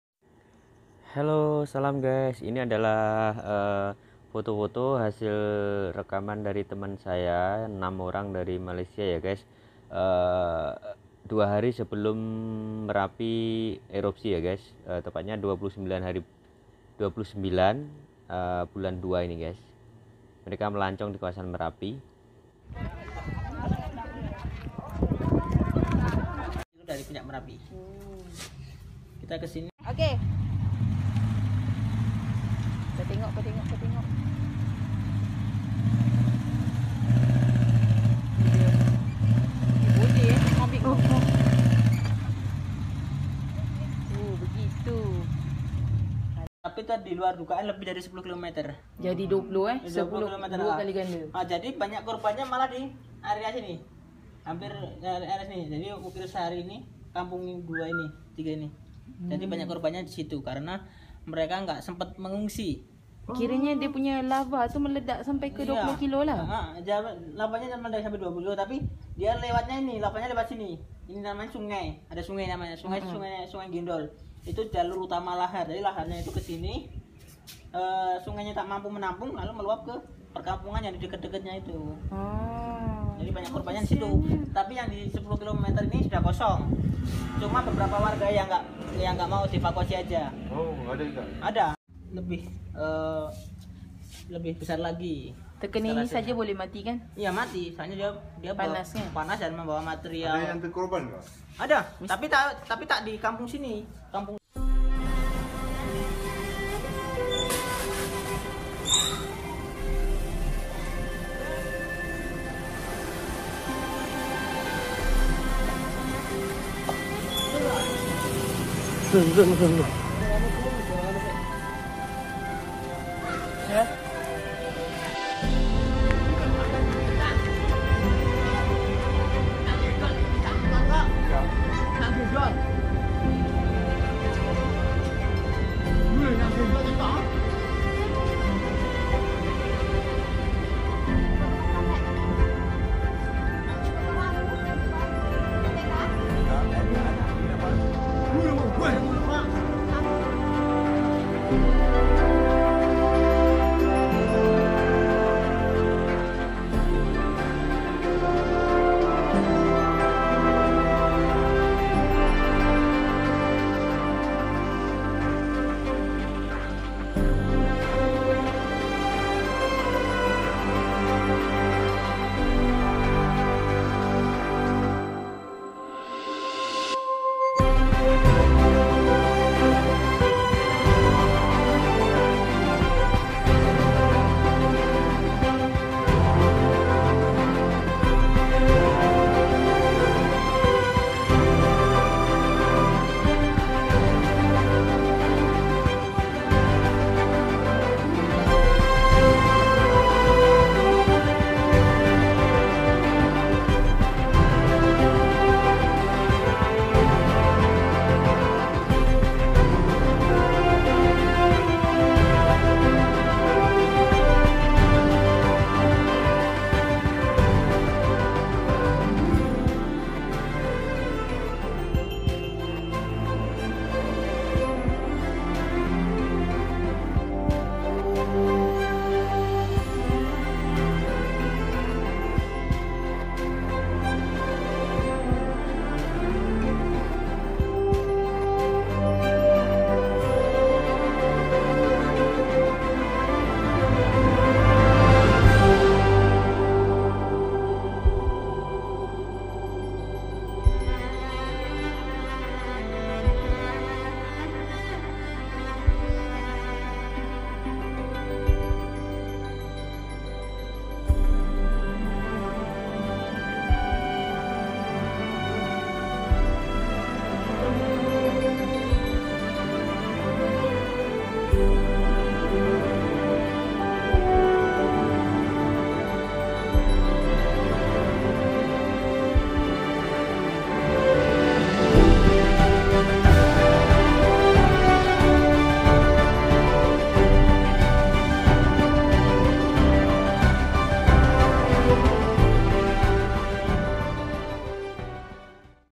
6 Warga negara MALAYSIA Berada di lereng MERAPI sebelum merapi erupsi
Sallam guys, jumpa lagi di vlog bebas, kali ni sya up, gambar dan vidio temen temen saya, yang berada di lereng merapi, 2 hari sebelum terjadinya erupsi ya guys,tepatnya 29/2 kemaren, Nasib baik mereka sudah turun & balek lagi ke malaysia,dan semoga sodara kita yang ada di lereng merapi senantiasa di lindungi oleh Allah SWT, smoga tidak terjadi korban jiwa, selamat menonton, smoga infonya memberi manfaat ,dan memberikan kesadaran, untuk senantiasa memohon perlindungan dari Allah atas semua cobaan dan bencana yang melanda negri kita